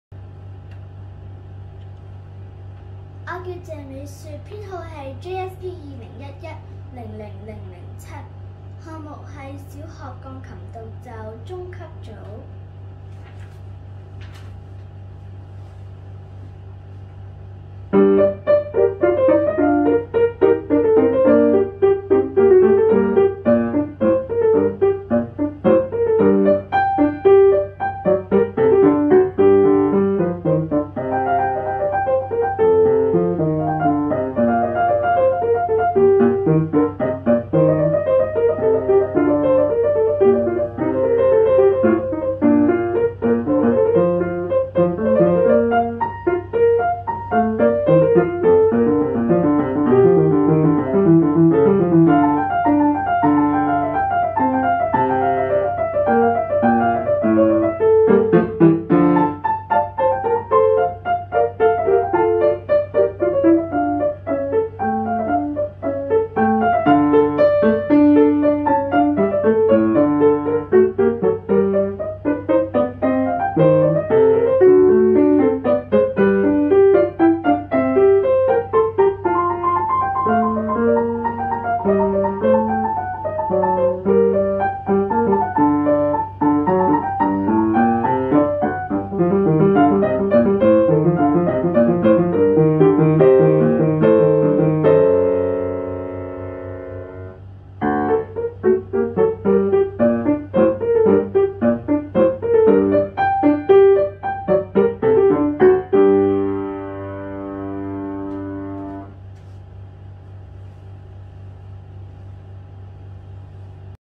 Music@e-Contest 2022 / 1st round / CAT:C / Piano / Mei Xue Mamie Tse (China)
Music@e-Contest 2022
The most popular international online music contest.
1st round competition
Repertoire:vivace
Composer:G.P.Telemann
--Biography--